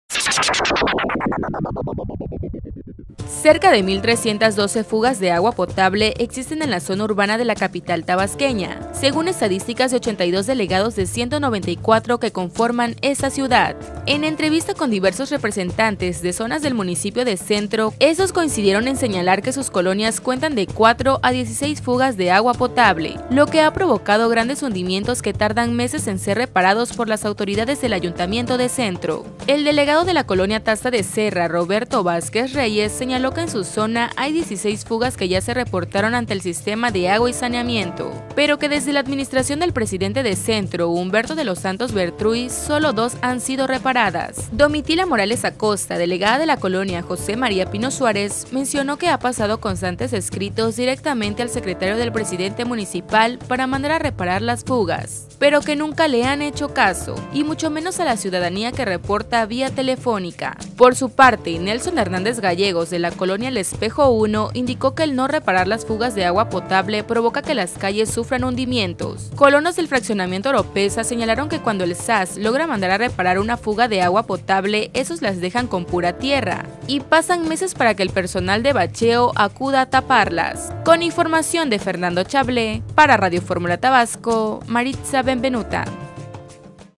312 fugas de agua potable en la zona urbana de Villahermosa
Cerca de mil 312 fugas de agua potable existe en la zona urbana de la capital tabasqueña, según estadísticas de 82 delegados de 194 que conforman esta ciudad.